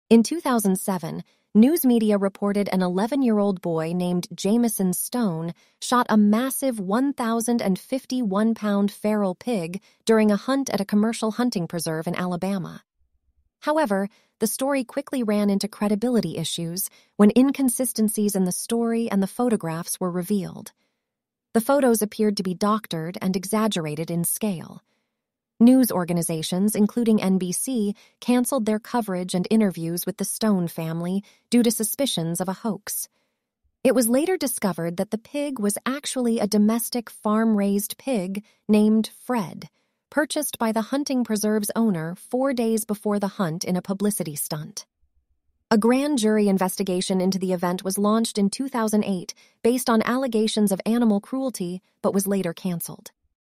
Truth Behind Hogzilla
In 2007, news media reported an 11-year-old boy named Jamison Stone shot a massive 1,051-pound feral pig during a hunt at a commercial hunting preserve in Alabama. However, the story quickly ran into credibility issues when inconsistencies in the story and the photographs were revealed. The photos appeared to be doctored and exaggerated in scale. News organizations, including NBC, canceled their coverage and interviews with the Stone family due to suspicions of a hoax. It was later discovered that the pig was actually a domestic farm-raised pig named "Fred," purchased by the hunting preserve's owner four days before the hunt in a publicity stunt. A grand jury investigation into the event was launched in 2008 based on allegations of animal cruelty but was later canceled.